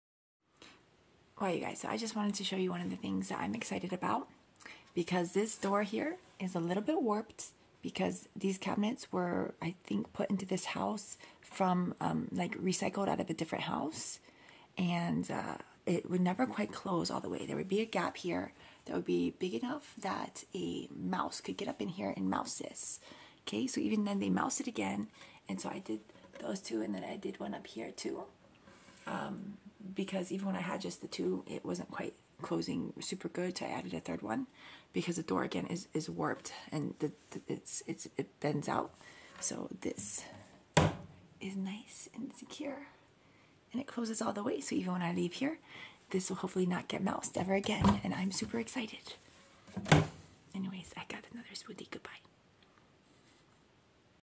magnet latches for my cabinet with the warped door woohoo
ordered these earlier in the winter and procrastinated, but happy to have it done